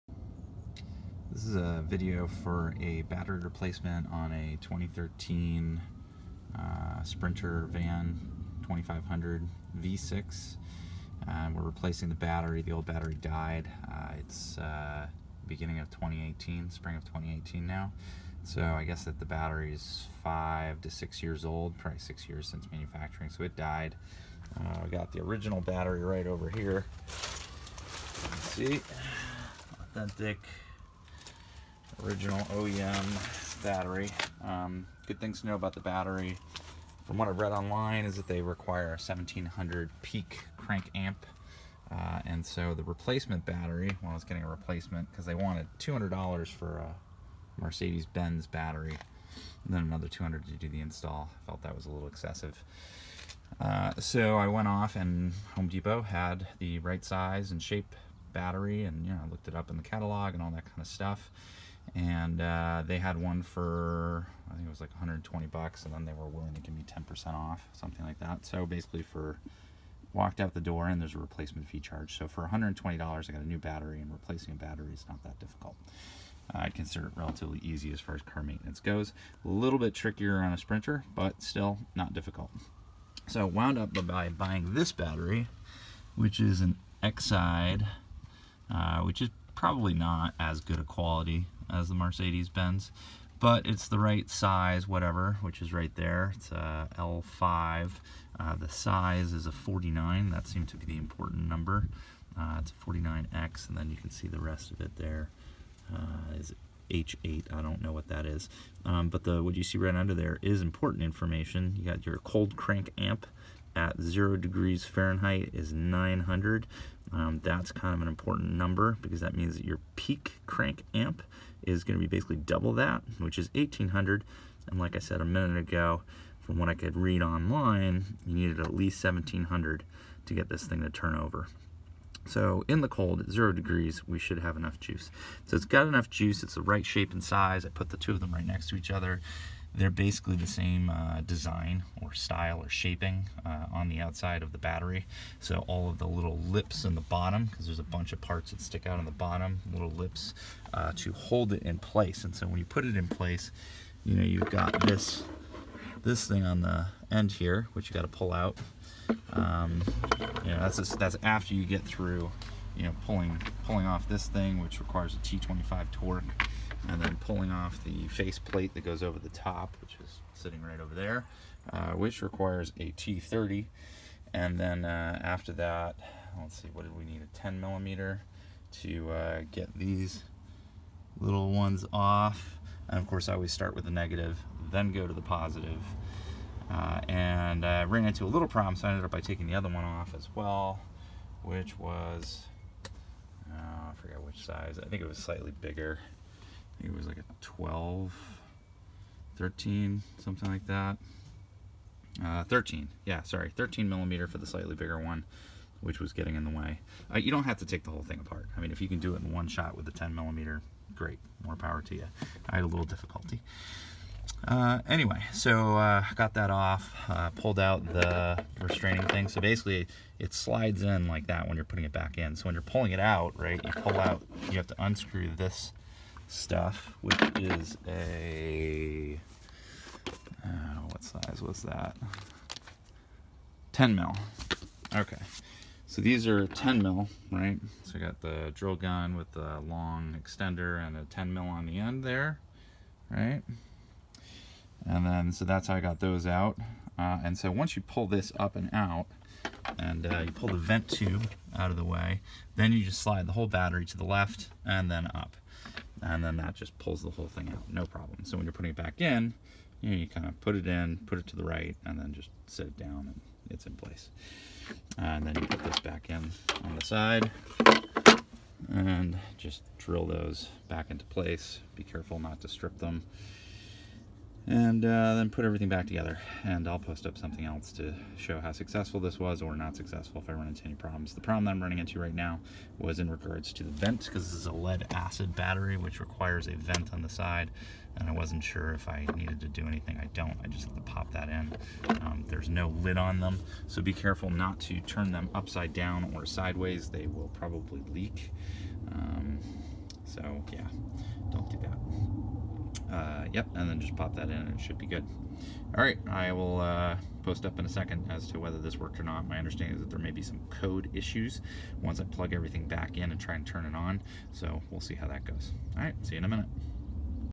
Battery replacement 2013 Sprinter V6
Success! There were no problems. The engine turned over a few times more than normal and the check engine light was on until it started, but after it started it went away and everything is working fine. No Code Error messages or anything else like that.

This is a MB 2013 Sprinter 2500 V6. The Battery is group/size 49, with a 900 Amp capacity.